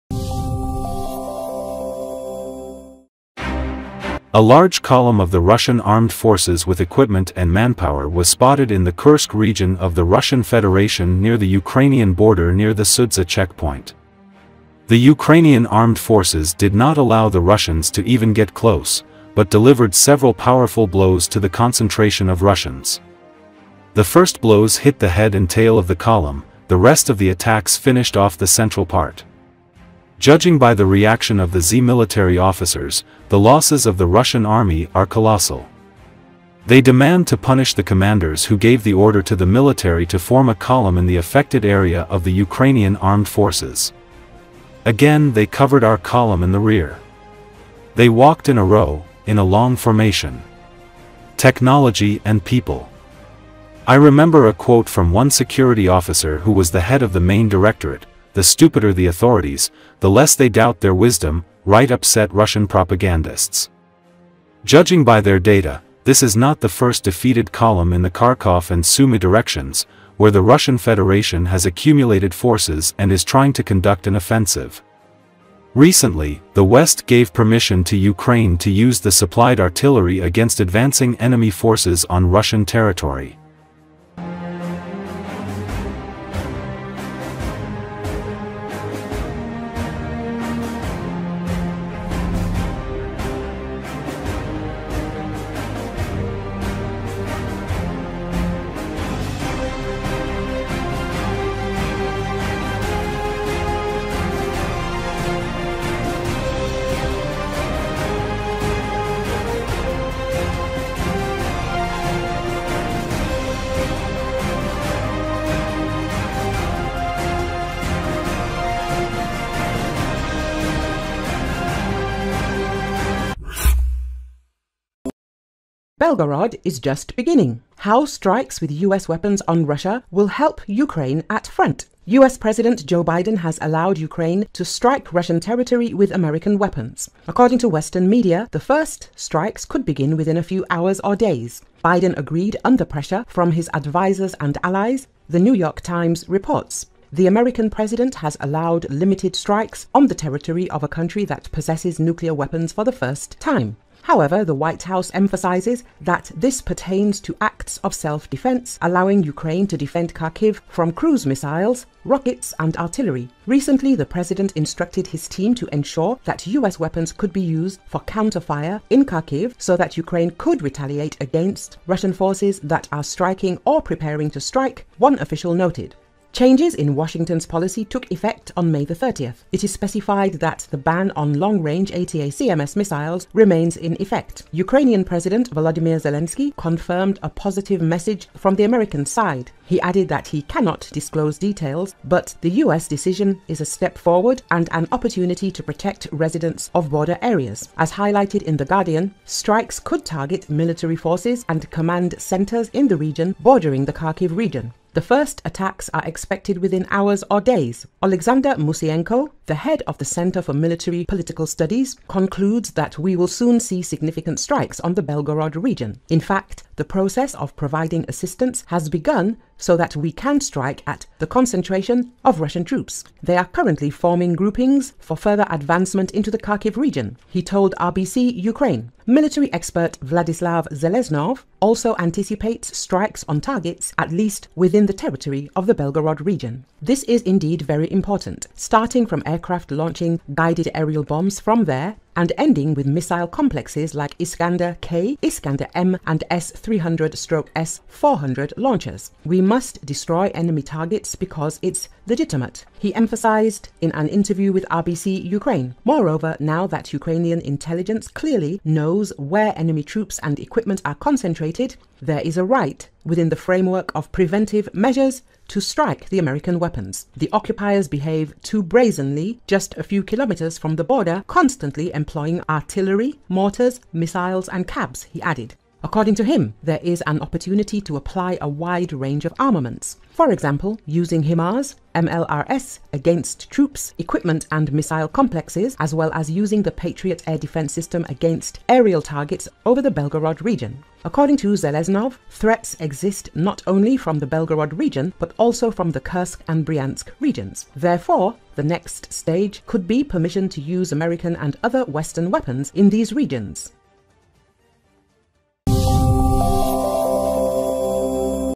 Convoy of dozens of equipments, preparing to enter to Ukraine, were fired upon in Russian territory
A large column of the Russian Armed Forces with equipment and manpower was spotted in the Kursk region of the Russian Federation near the Ukrainian border near the Sudzha checkpoint. The Ukrainian Armed Forces did not allow the Russians to even get close, but delivered several powerful blows to the concentration of Russians. The first blows hit the head and tail of the column, the rest of the attacks finished off the central part.
Judging by the reaction of the Z-military officers, the losses of the Russian army are colossal. They demand to punish the commanders who gave the order to the military to form a column in the affected area of the Ukrainian Armed Forces.
“Again they covered our column in the rear. They walked in a row, in a long formation. Technology and people. I remember a quote from one security officer who was the head of the Main Directorate: “The stupider the authorities, the less they doubt their wisdom,” write upset Russian propagandists.
Judging by their data, this is not the first defeated column in the Kharkov and Sumy directions, where the Russian Federation has accumulated forces and is trying to conduct an offensive. Recently, the West gave permission to Ukraine to use the supplied artillery against advancing enemy forces on Russian territory.